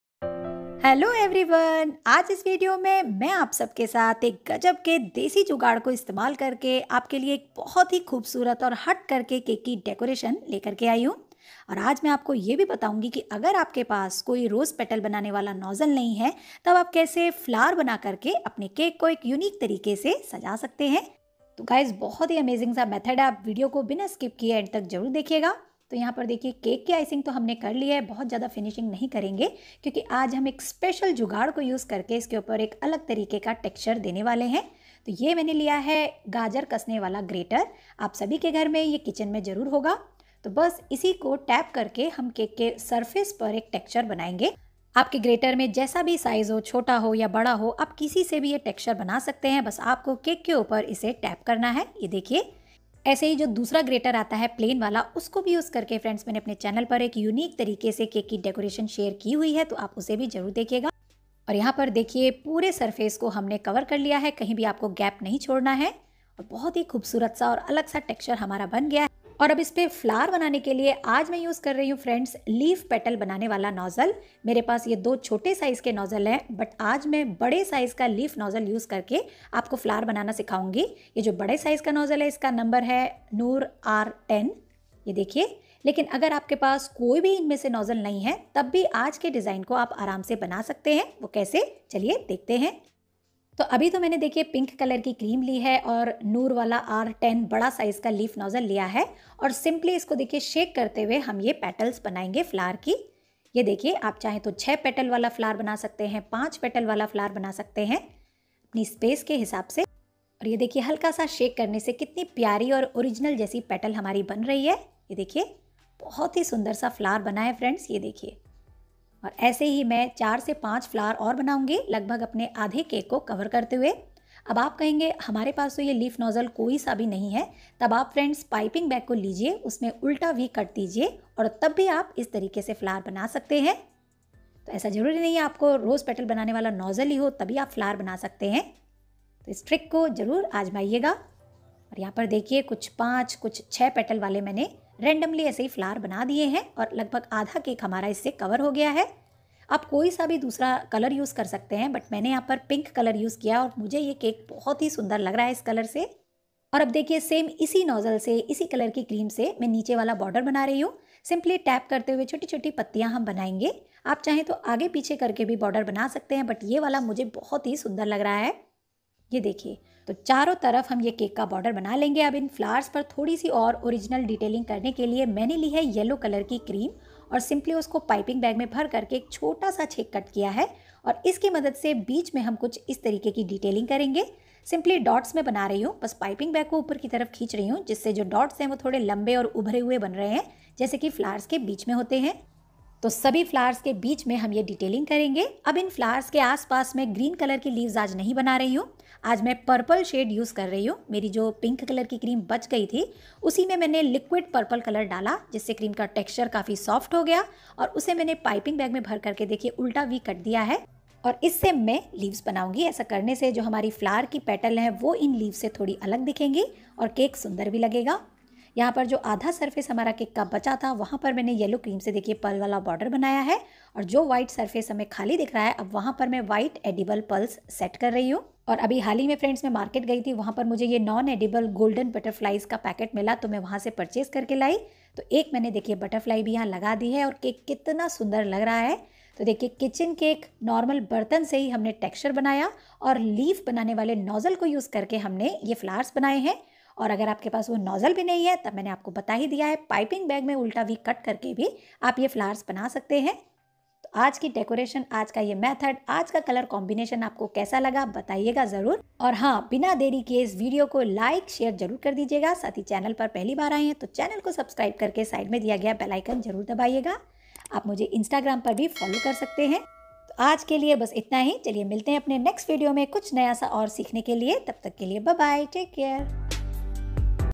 देसी जुगाड़ से सजाये cake.New Easy trick for cake decoration . Trending Birthday Cake decoration.
For more recipes related to baking, theme cake, gel cake, cake decoration ideas,  fondant, whipped cream,  etc on my channel please check playlist below:-